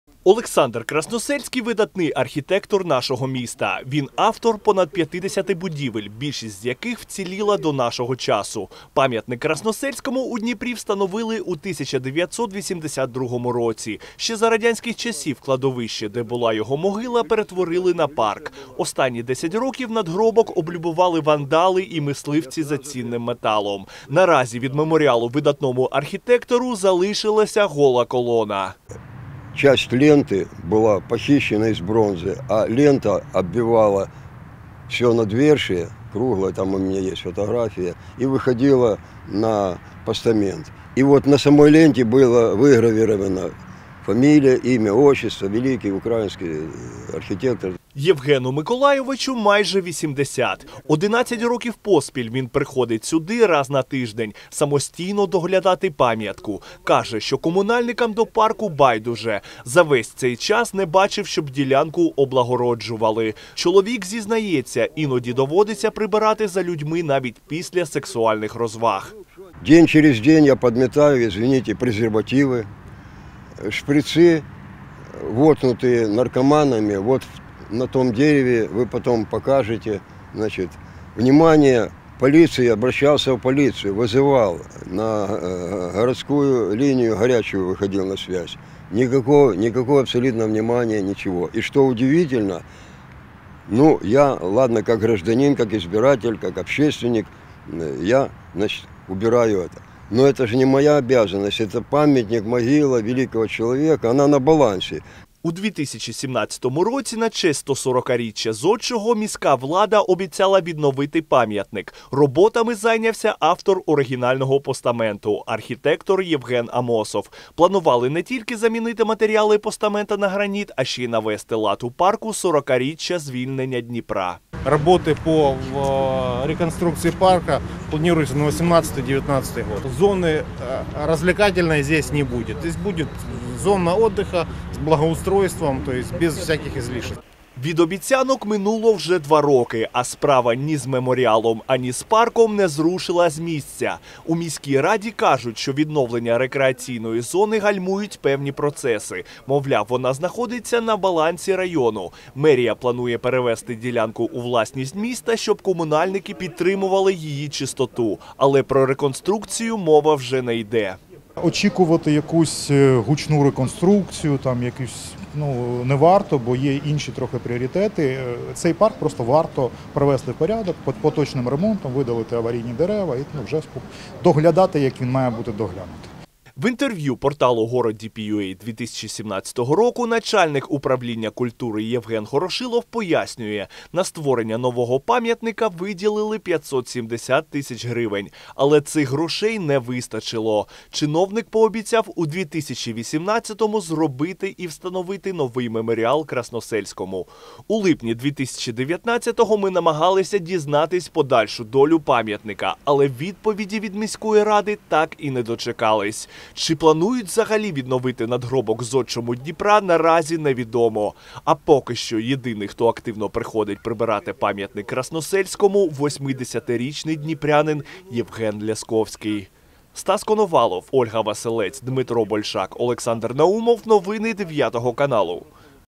Коли ж відреставрують пам'ятник архітектору Красносельському?
До дня 140-річчя Олександра Красносельського, міська влада обіцяла відновити пам'ятник архітектору нашого міста. За останні 10 років меморіал зодчому Дніпра розграбували мисливці за цінним металом та спотворили вандали. Наразі від надгробку залишилася гола колона. Проте, з часу обіцянок минуло вже два роки, а справа не зрушила з місця. Журналісти 9-го каналу намагалися розібратися у цій ситуації. Далі у сюжеті.